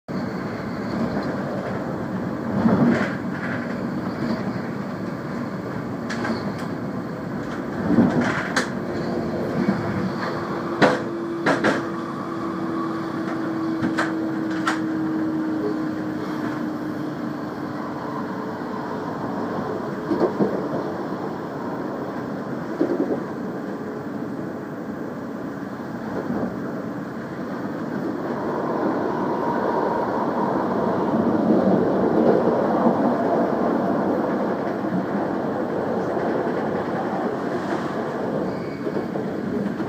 【スイス】ビール(ビエンヌ)湖もきれい！【スイス情報.com】
スイス・ベルン州の2ヶ国語の町、ビール(ドイツ語)・ビエンヌ(フランス語)にある湖も素敵です！